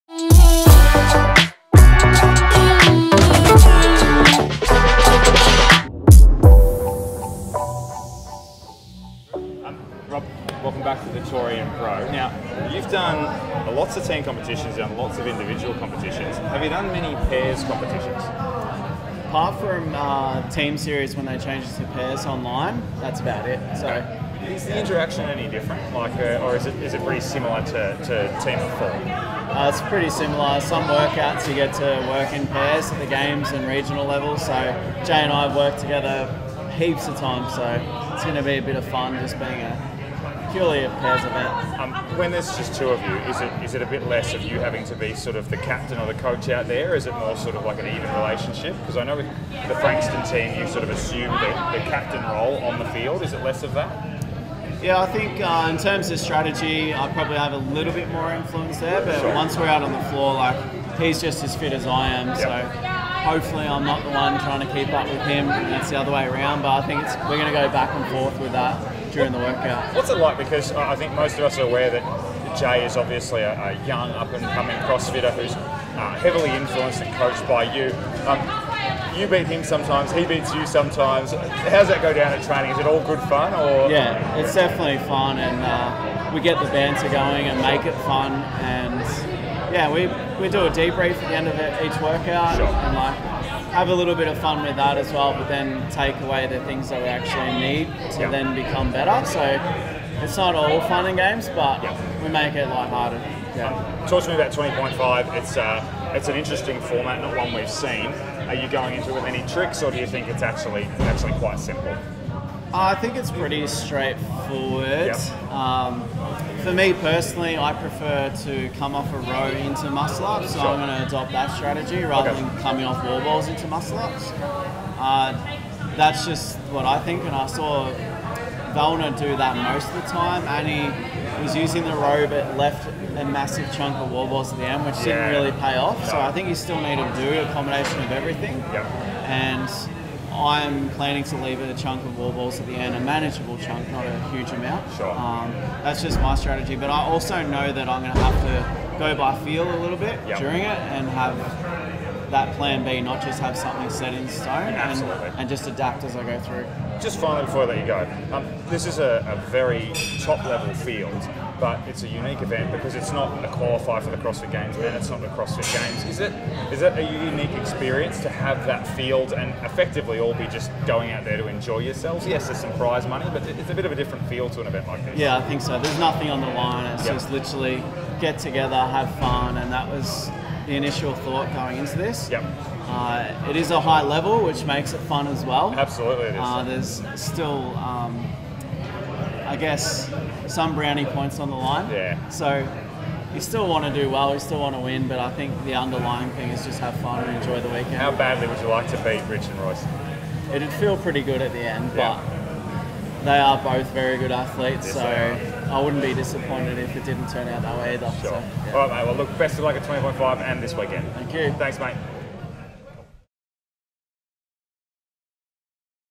TWL Interview: Rob Forte
Ed Morrison from Pounds to Kilos takes you through the Torian Pro 2019, the premier functional fitness festival in Australia.

The following is an interview he had with Rob Forte at the event.